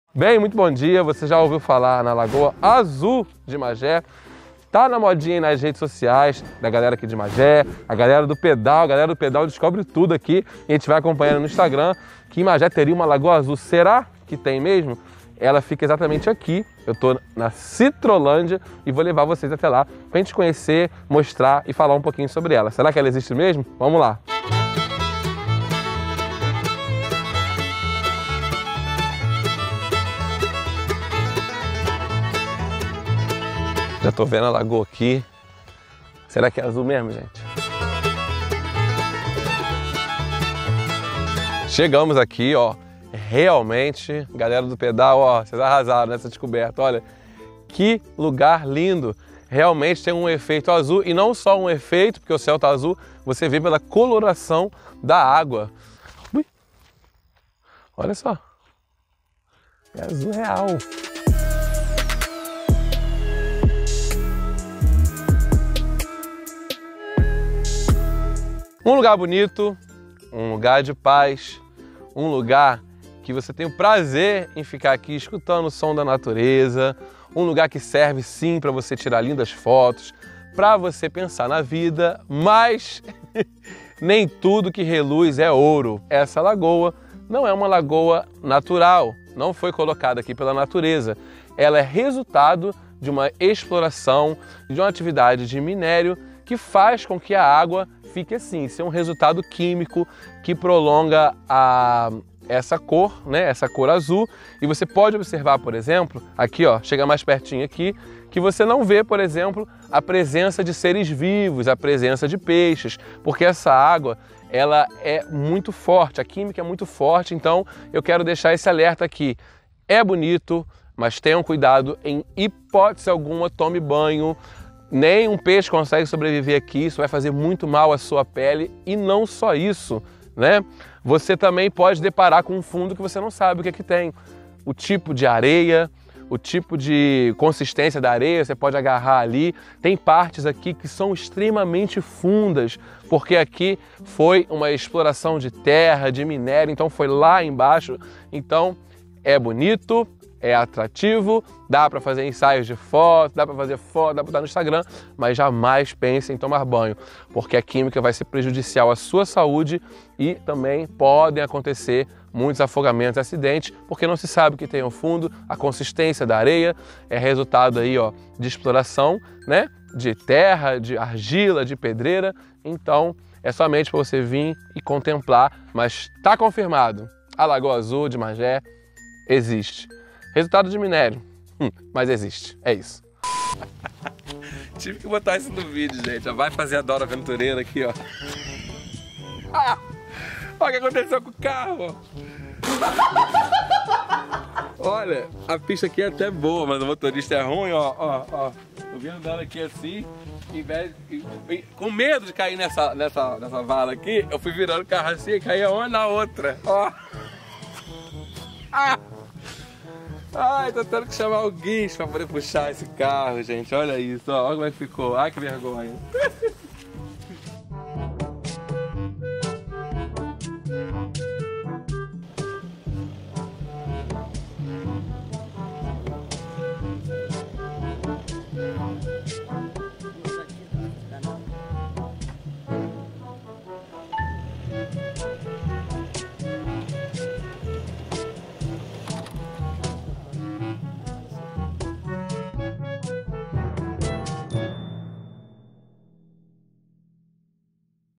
Lagoa azul de Magé
conheça a lagoa azul de Magé
fica no bairro citrolandia é resultado de exploração de minério.
apropriada para tirar boas fotos mas não é indicada ao banho.